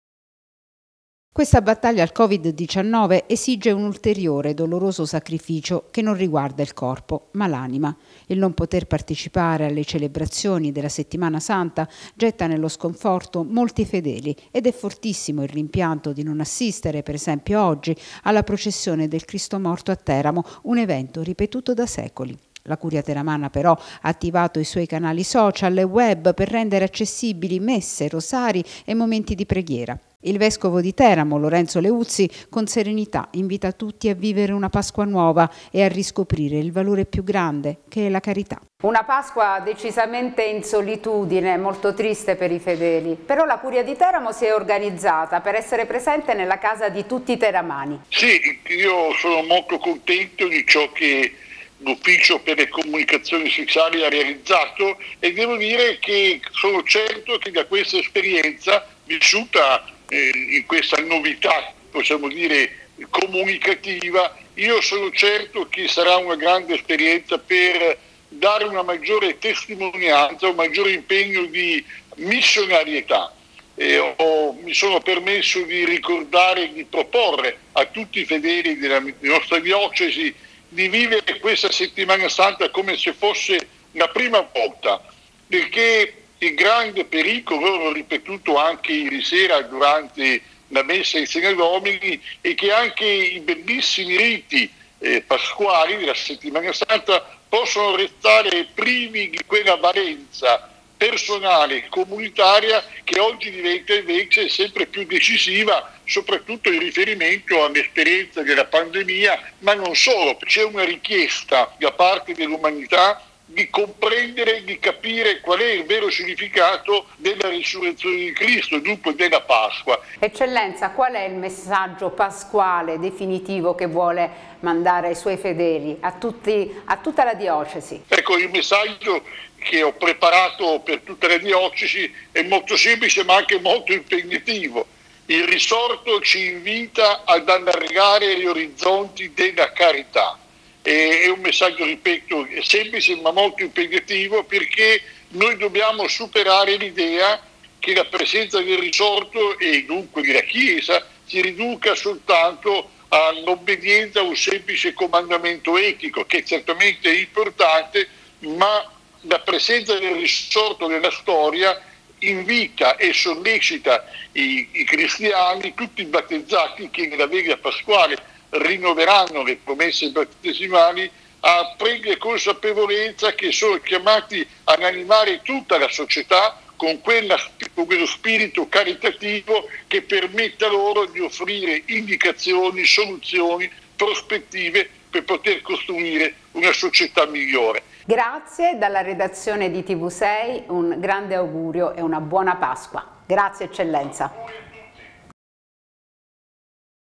La Curia di Teramo trasmetterà le celebrazioni pasquali su web e tv
Il vescovo della diocesi di Teramo-Atri, monsignor Lorenzo Leuzzi, invia un messaggio pasquale a tutti i fedeli teramani: "Siate forti, vivete una nuova Pasqua e riscoprite la carità". Intanto la Curia teramana si organizza e annuncia che tutte celebrazioni della Settimana Santa saranno trasmesse su web e tv